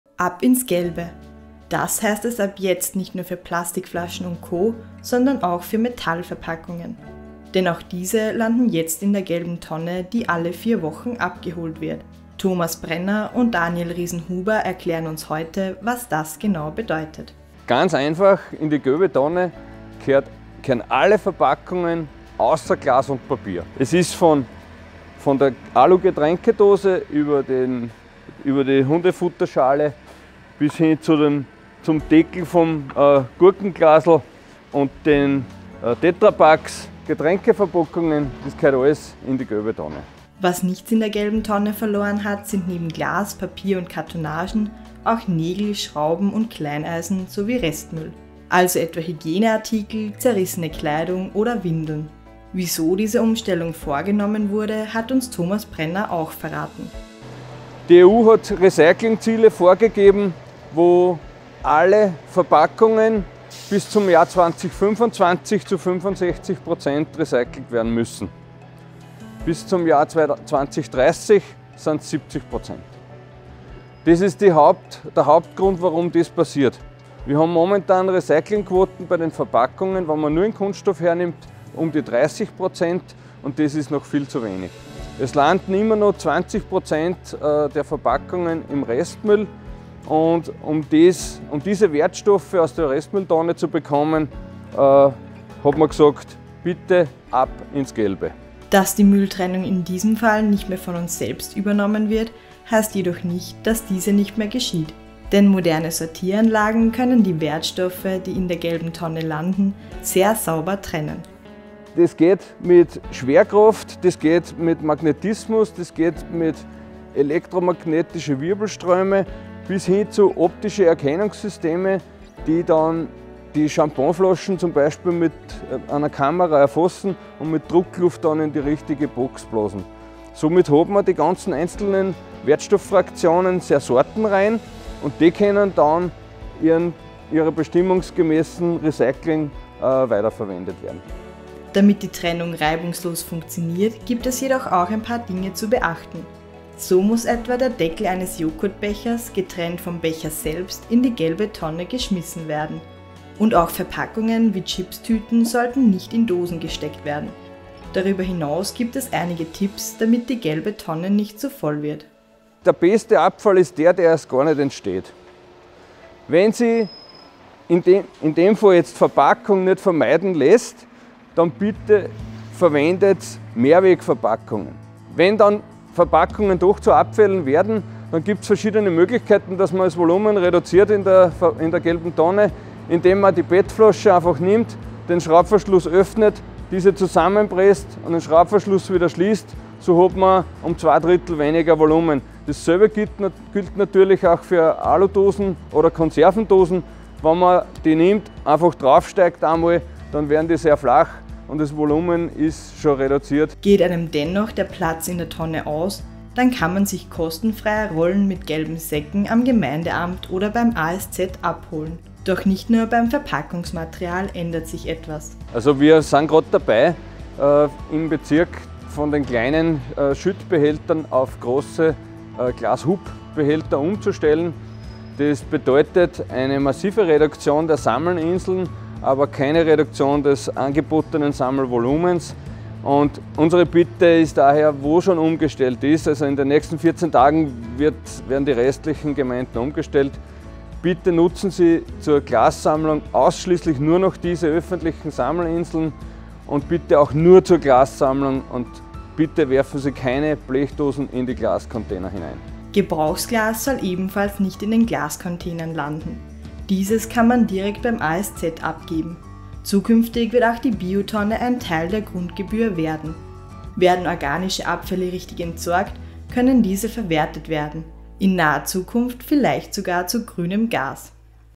Ab ins Gelbe: So funktioniert die Mülltrennung ab jetzt richtig!
Ab ins Gelbe - das gilt jetzt auch für Metallverpackungen. Wir haben die GVU Scheibbs besucht und nachgefragt: Was genau kommt jetzt eigentlich in die gelbe Tonne? Was darf nicht hinein? Und wieso kam es überhaupt zu dieser Umstellung? Thomas Prenner und Daniel Riesenhuber haben uns Antworten auf diese Fragen gegeben.